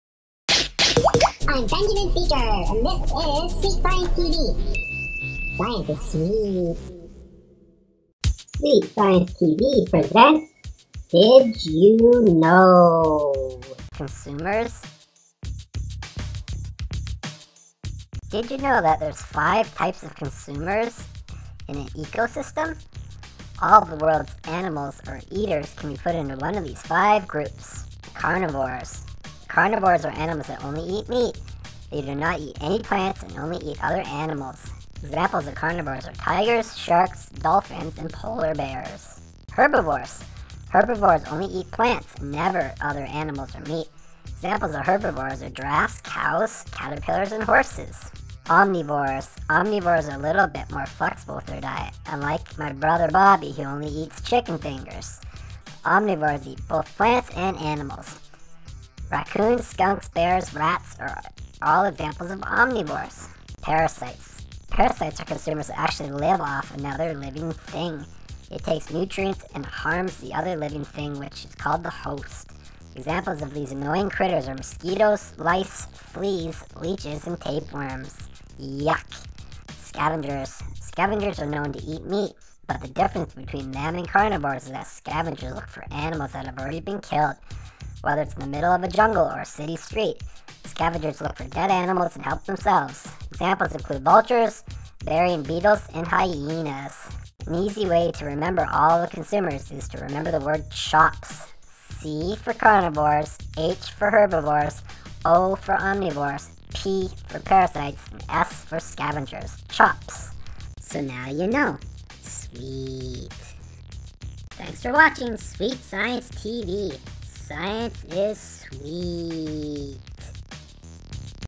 Did You Know - Consumers
Benjamin Beaker teaches you the five different types of consumers that live in an ecosystem!